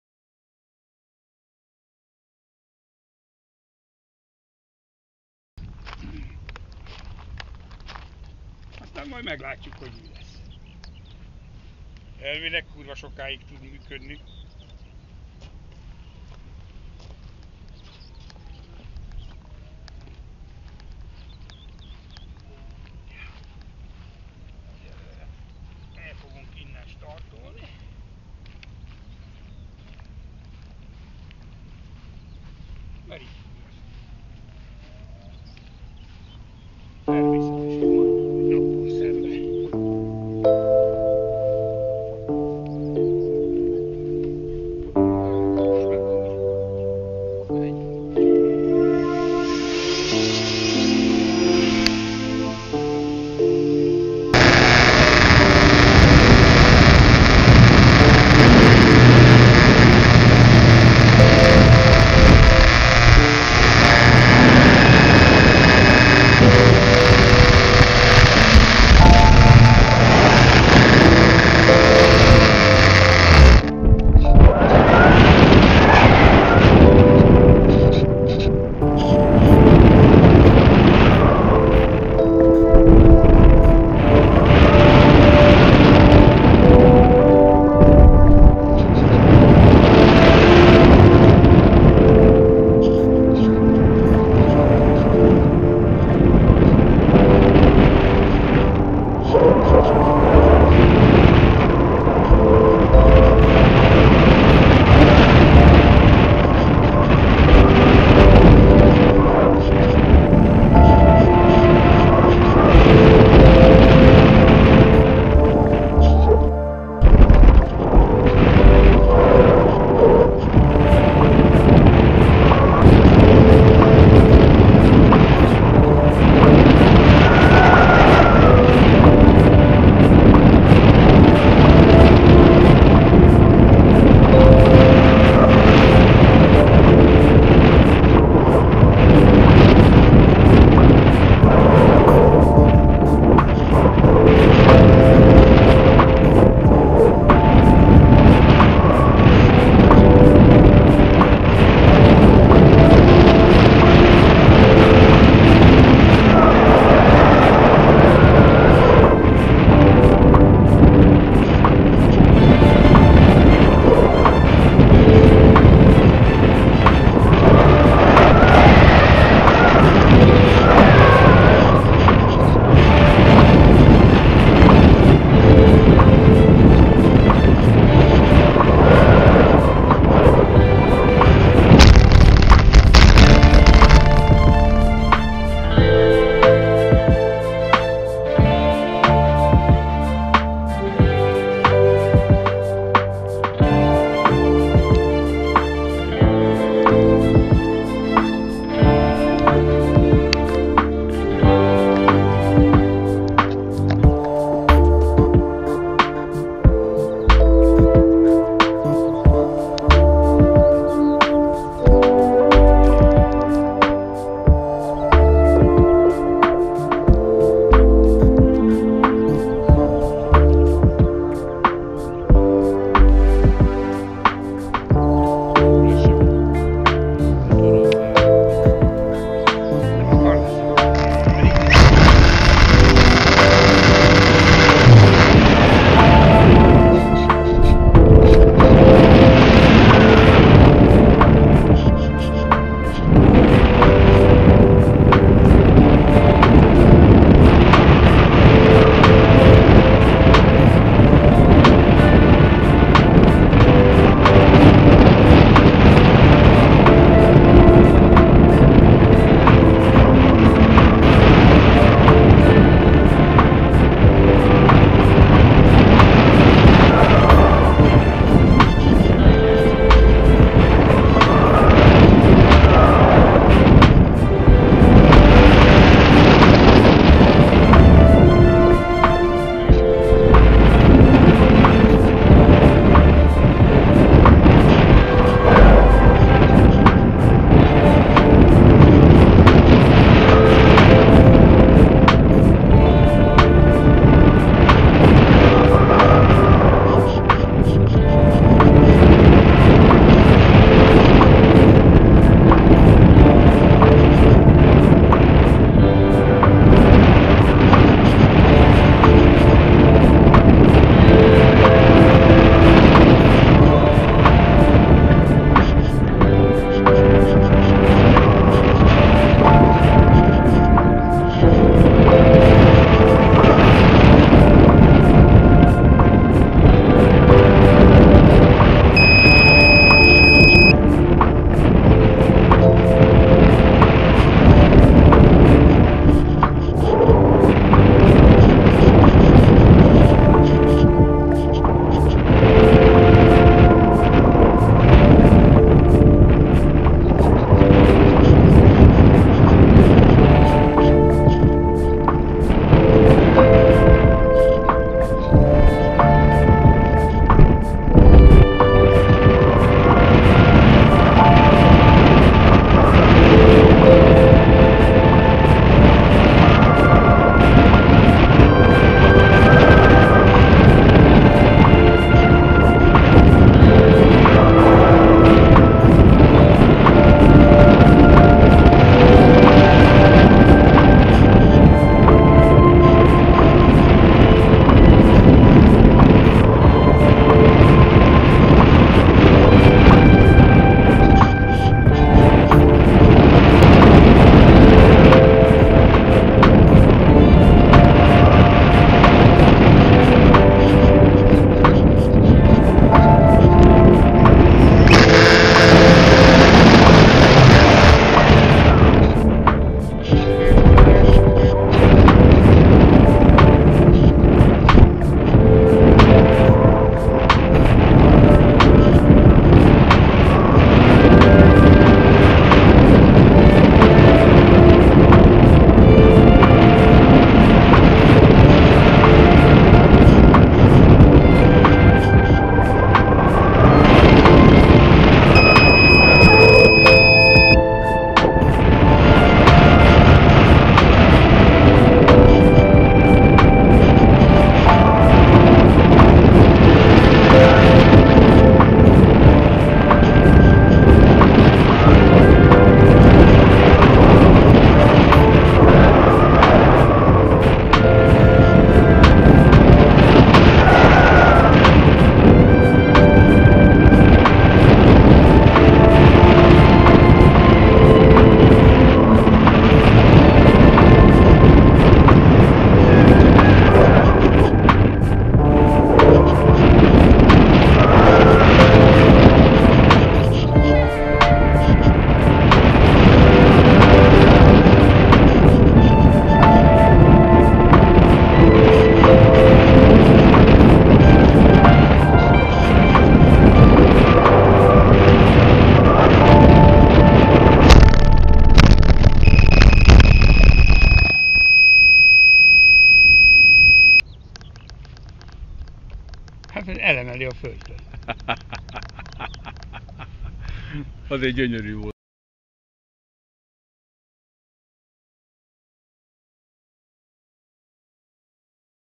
R.K.V.t.
Sokadik kamera próba, de már lassan alakul.
Az időjárás  remek volt,   7-9 óra  között repkedtünk, utána  már  bazi meleglett a réten.